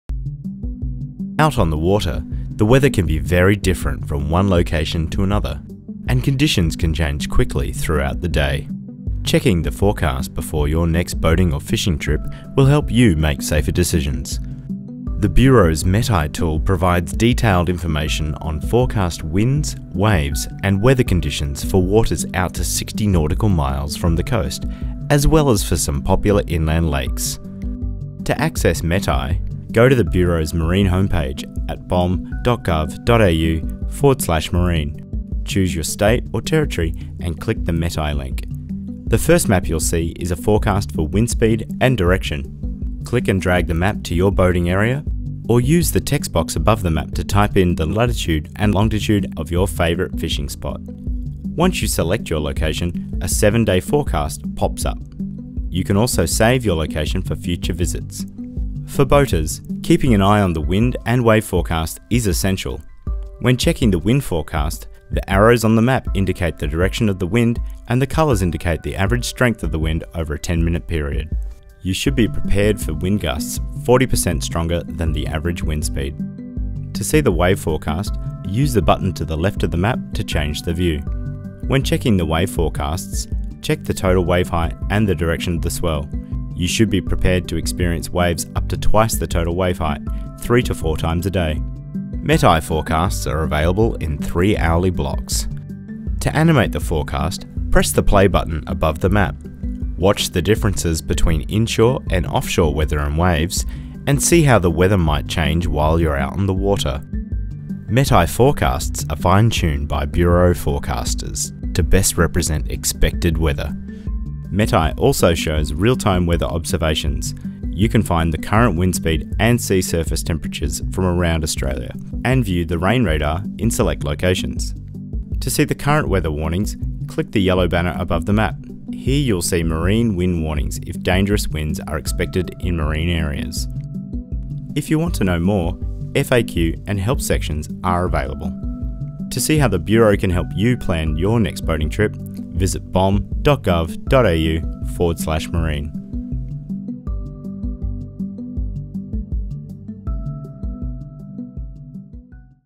MetEye - for marine and boating
MetEye is able to show maps of weather observations and the official forecasts produced by the Bureau of Meteorology. This is quite different to other map based systems which often show automatically generated, computer model data. MetEye forecast maps have been adjusted by our meteorologists to better represent expected weather. This video explains how Mariners and boaters can use MetEye to prepare before they head out onto the water.